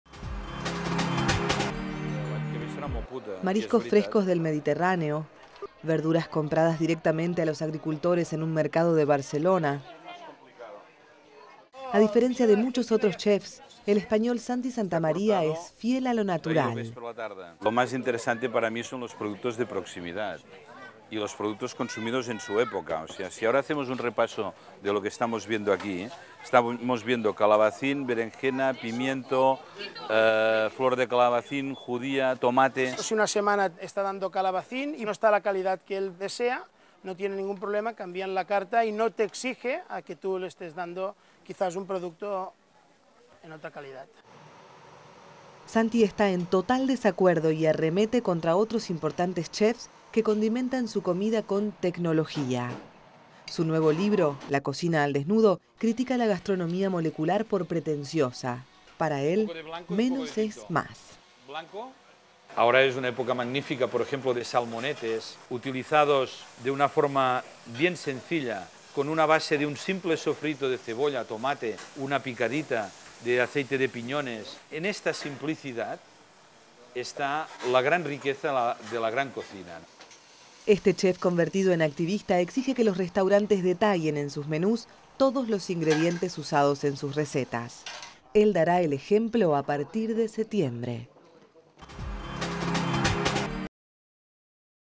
Chef de Barcelona enfrenta gastronomía natural vs molecular
La gastronomía molecular es una tendencia popular entre gourmets. En el menú aparecen artículos exóticos como el nitrógeno líquido. Pero Santi Santamaria, destacado Chef español que entre sus tres restaurantes tiene seis estrellas Michelin, está contra lo que considera como superfluo y pretencioso. Él apuesta a una vuelta a lo natural.